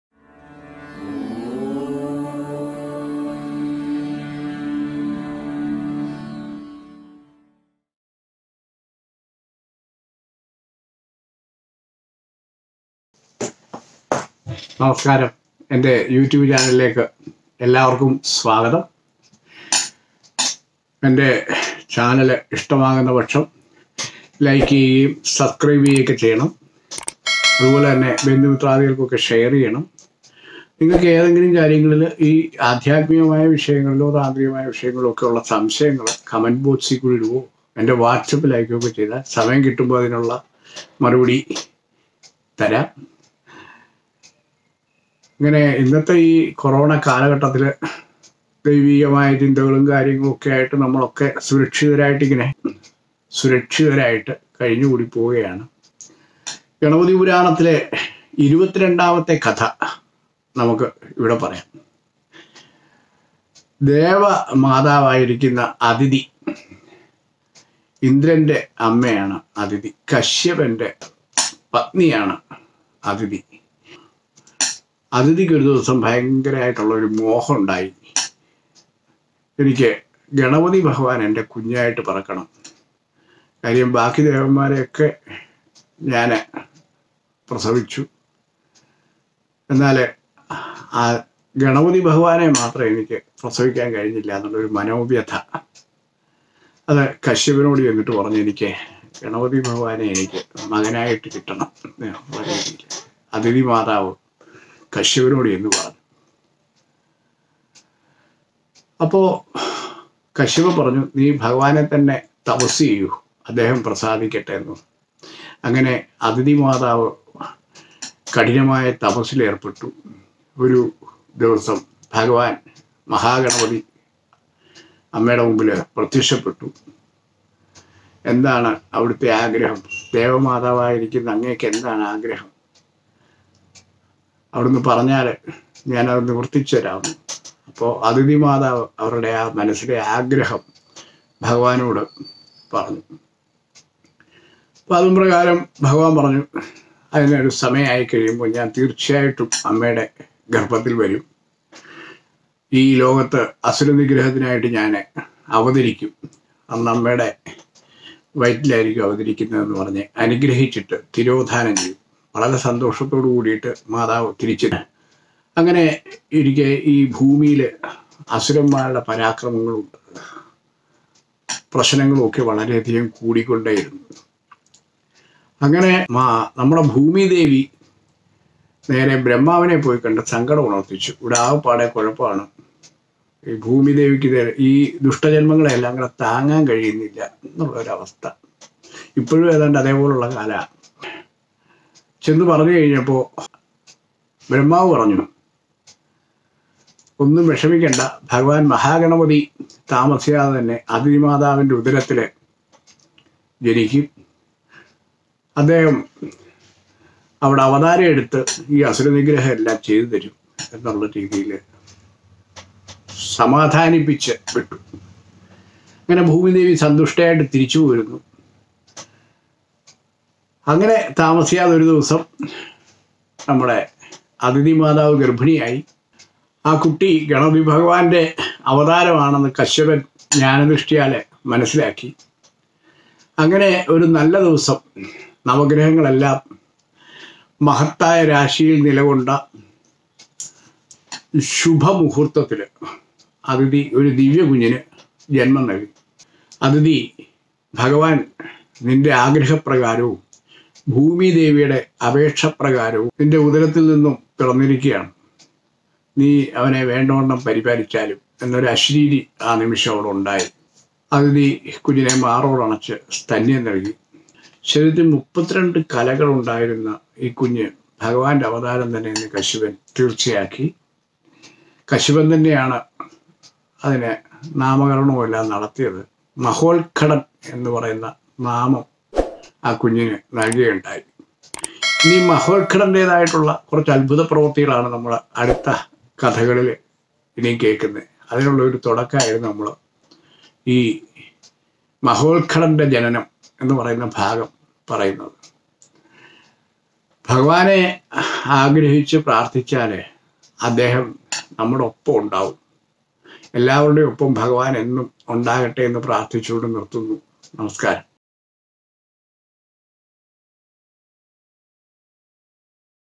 ദൈവമാതാവിന്റെ ആഗ്രഹം II GANAPATHI PURANAM 22 II Thanthri Dileepan Narayanan Nampoothiri
ഗണപതി ഭഗവാനെ പുത്രനായി ലഭിക്കാൻ ദേവമാതാവായ അദിതി തപസ്സനുഷ്ടിക്കുന്നു.

Tech Travel Eat by Sujith Bhakthan Marunadan TV @Hidden Facts